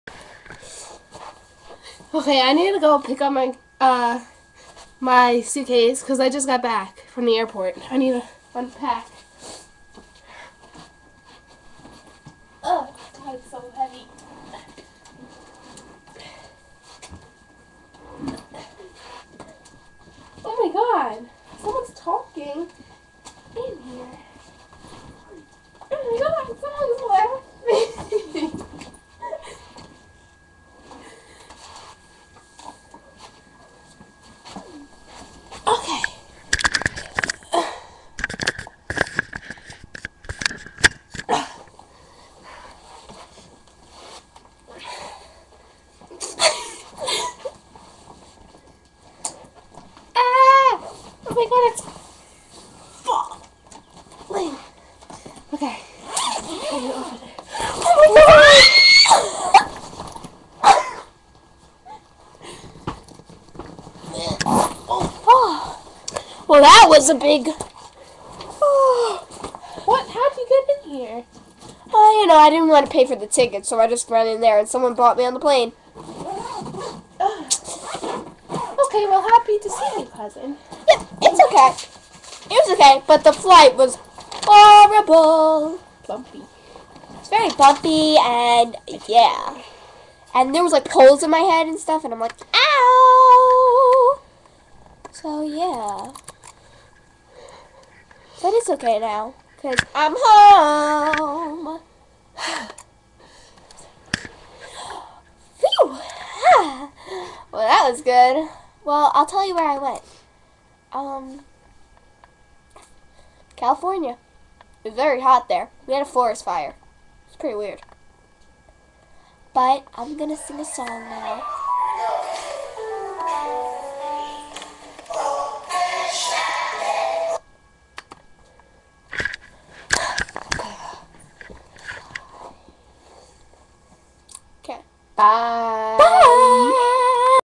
mollycaitlin123- supriseee in my suitcase.!
i seriously actually didd just get back from the airport and i guess my mom threw out all my clothess and put my cousin in it and i really didnt know she was in there.! and suprisingly my other cousin was recording me becasue she told me to open the suitcase she bought it downstairs.